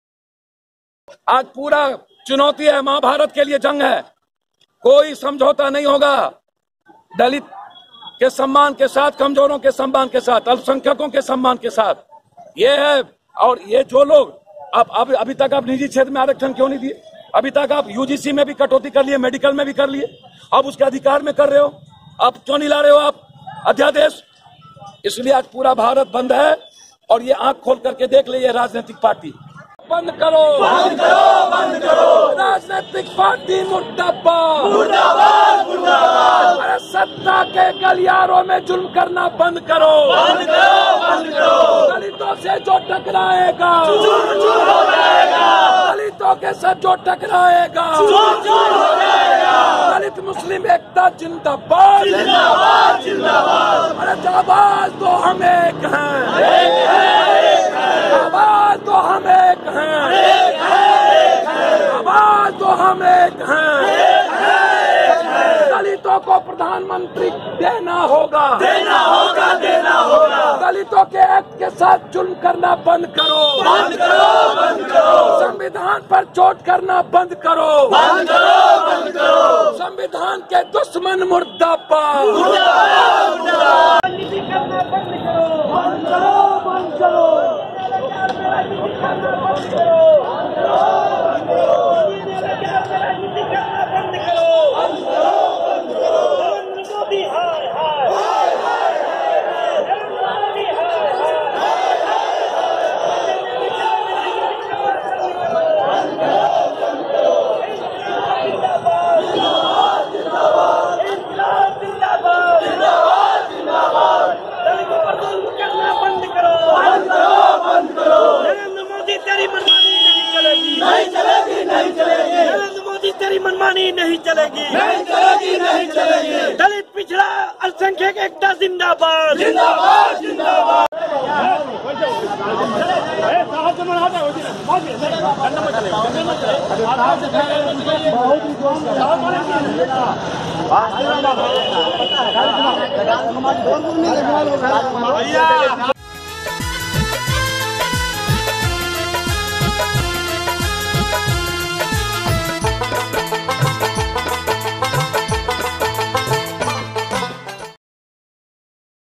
भारत बंद के समर्थन में सड़क पर उतरे पप्पु यादव | धुँआधार नारा | खतरनाक बयान | छा गये..
भारत बंद का महाकवरेज-
भारत बंद के समर्थन में सड़क पर उतरे पप्पु यादव, दिया दलित-मुस्लिम एकता जिंदाबाद का नारा..